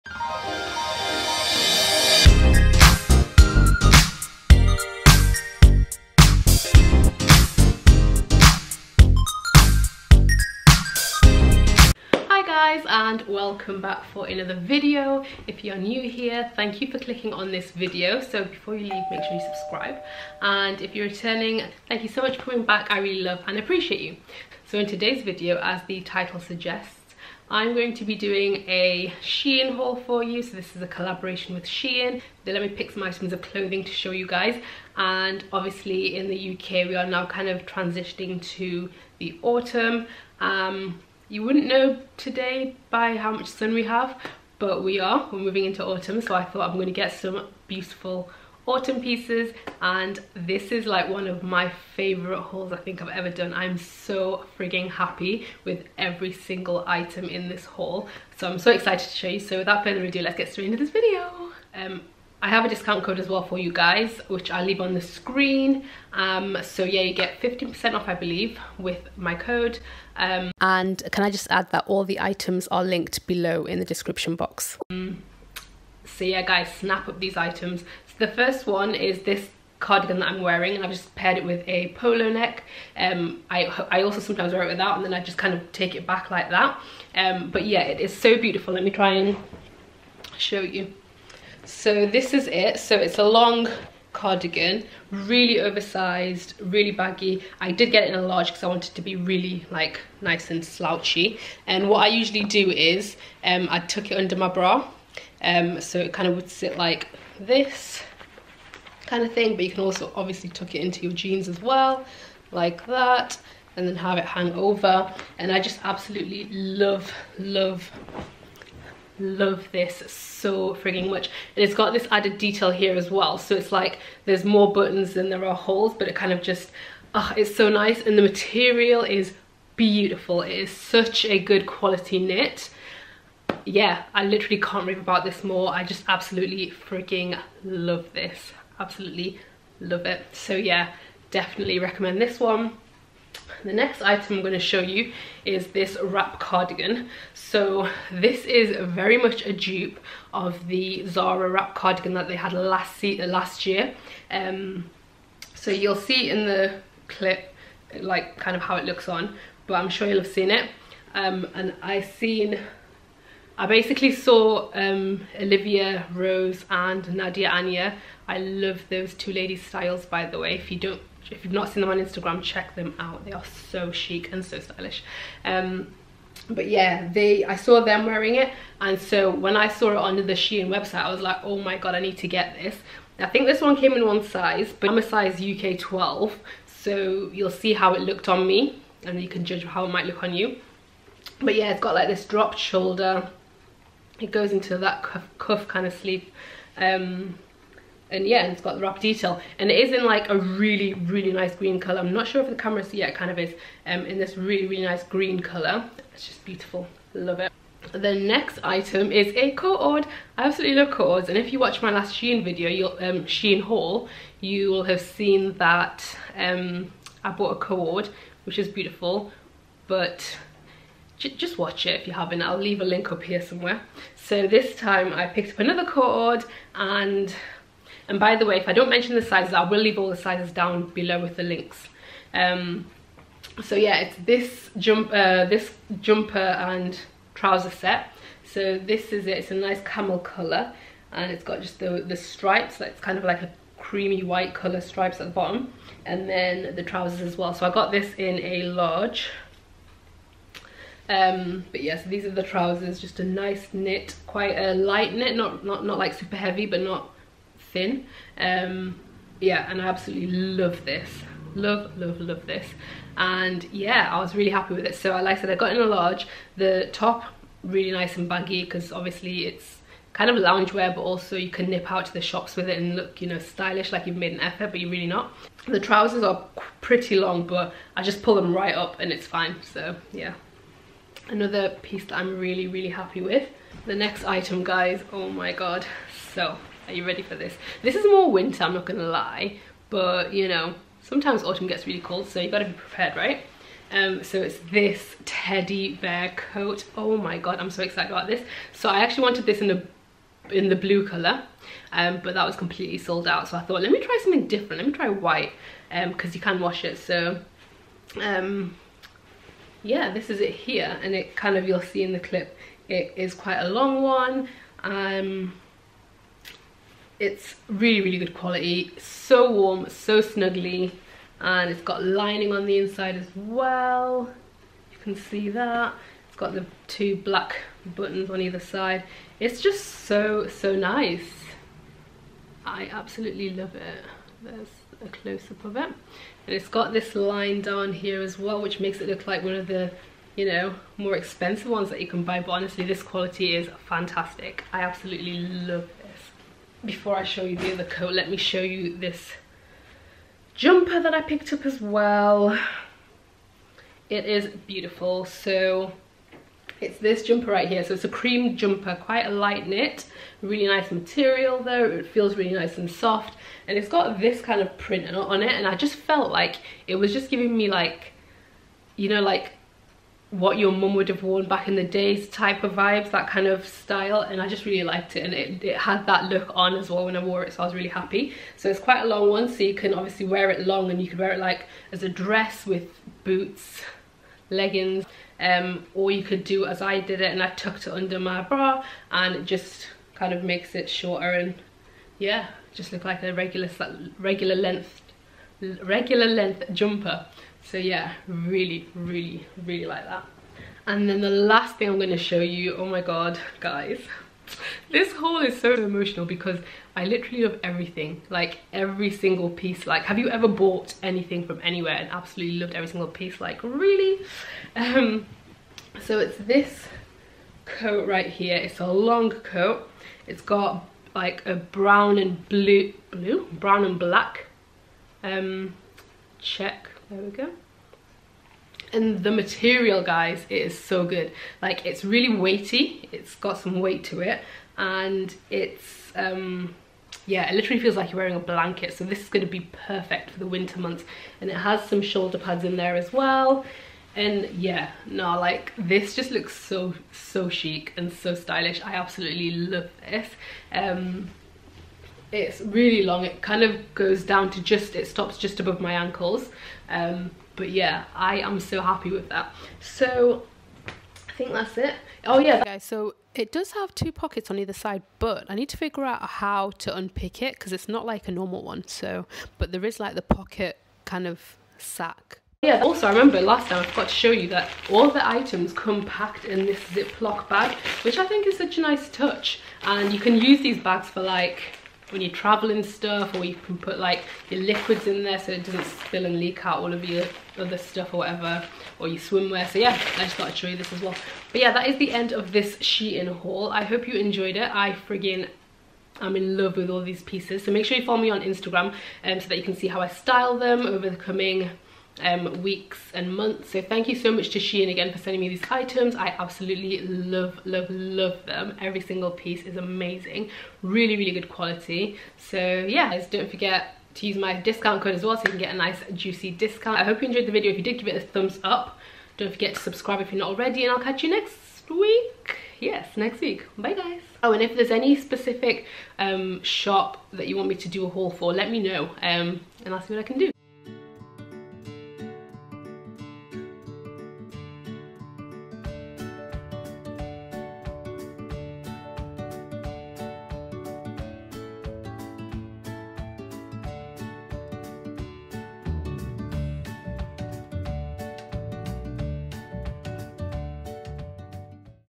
SHEIN AUTUMN (FALL) TRANSITIONAL HAUL | TRY ON | SEPTEMBER 2020 *Discount Code*
I have a Shein Autumn transitional haul featuring some amazing finds for the fall! It features, chic knitwear, coats, including a teddy bear coat, an amazing Zara dupe, and more. I have a discount code for you all to use to get 15% off your order using my code ‘2aishcream15’.  Shein is a super affordable online store where you can get pretty much any type of clothing that you could be after. Product links are listed below. Let me know which look is your favourite. xoxo

All items were gifted. All opinions are entirely my own.

FOR REFERENCE:I am a UK size 12/Medium
My height 5’5”
Shoe size 6 (UK)
I will leave the sizes that I got beside each item:

Long Plaid Coat (L)

Green wrap front cardigan (One size)

Co-ord set (L)

Turtle neck oversized jumper (L)

Burnt Orange oversized cardigan (L)

Teddy Coat (L) (currently sold out)
Similar options:

Enjoy xoxo

 🎞Tech! What I film With
Canon G7x Mark II

Stay blessed xo